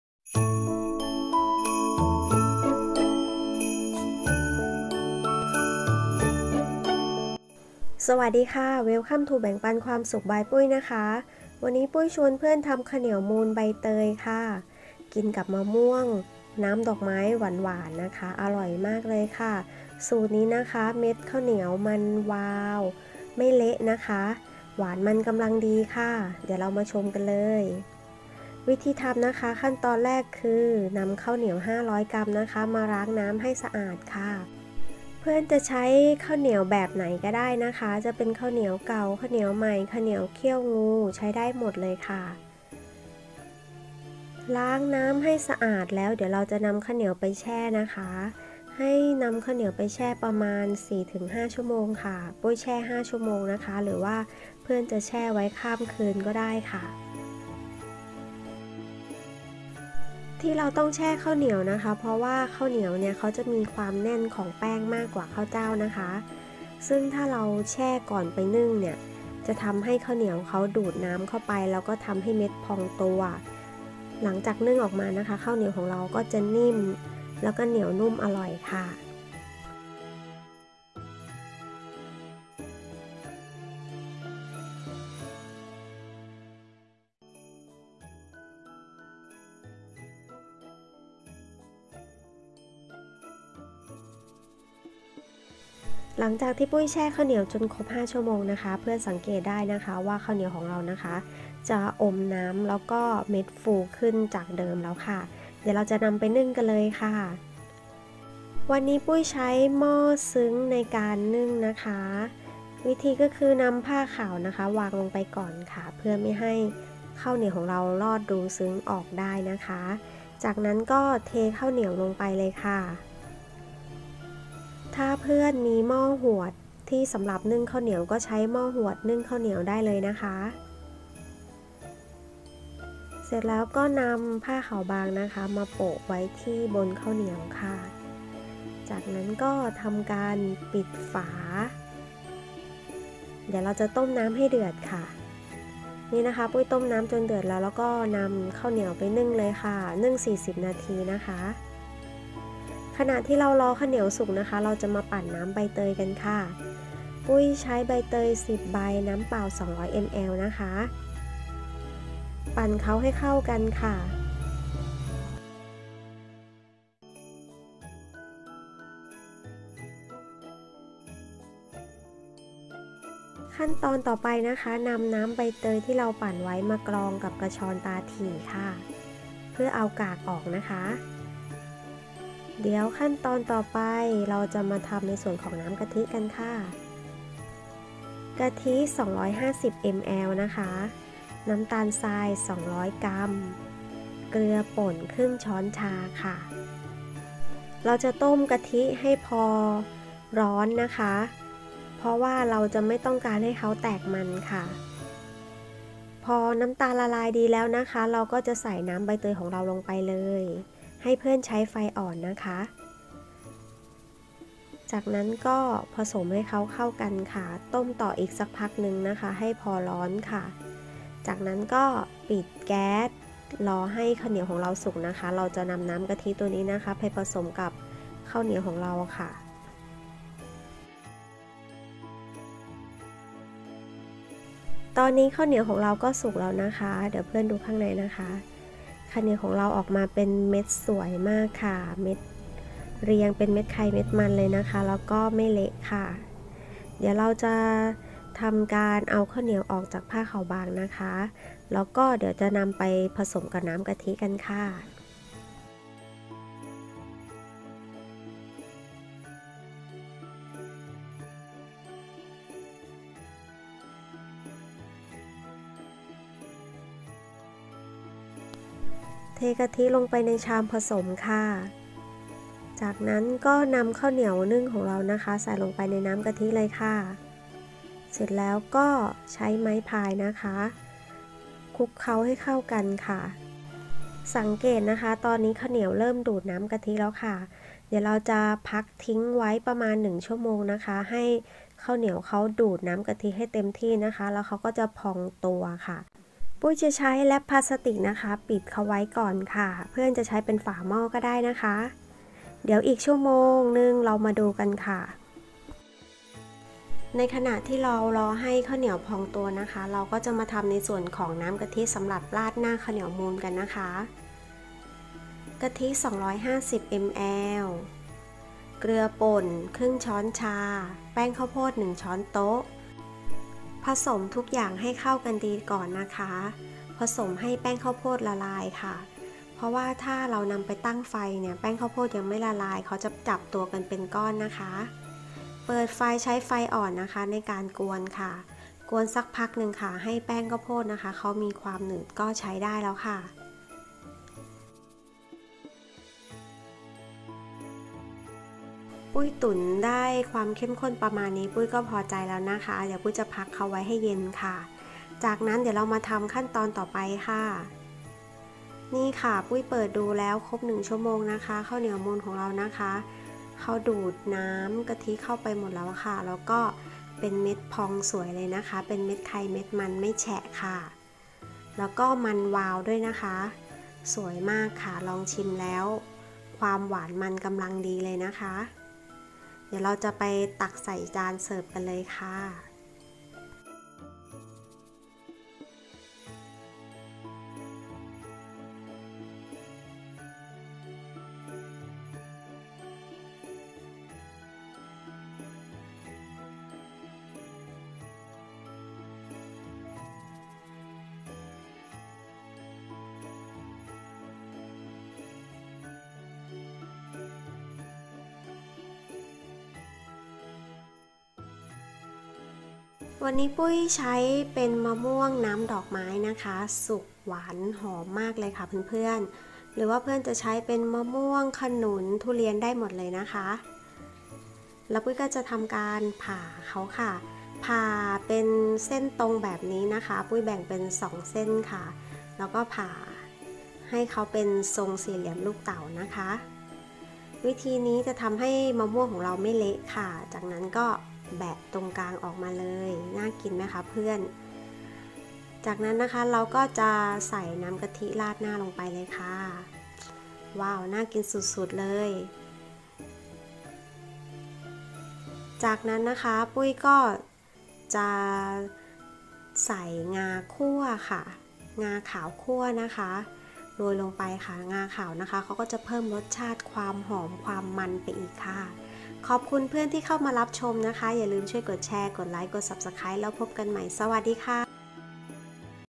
ข้าวเหนียวมูนใบเตยมะม่วงอร่อยหวานมันไม่เละ Thai Sweet Sticky rice and manga
ข้าวเหนียวมูนใบเตยกินกับมะม่วงอร่อยมากอร่อยหอมมันหวานกำลังดีวิธีทำง่ายมาก
ข้าวเหนียว 500 กรัม
กะทิ 250 ml
น้ำตาลทราย 200 กรัม
เกือบ 1/2 ช้อนชา
ใบเตย 10 ใบ
น้ำเปล่า 200 ml

ส่วนผสมกะทิราดหน้าข้าวเหนียวมูน
แป้งข้าวโพด 1 ช้อนโต

ingredient
500 g sticky rice
coconut milk 250 ml
200 g sugar
1/2 tsp Salt
10 pandans

coconut sauce
1/2 tsp Salt
1 tbsp Corn flour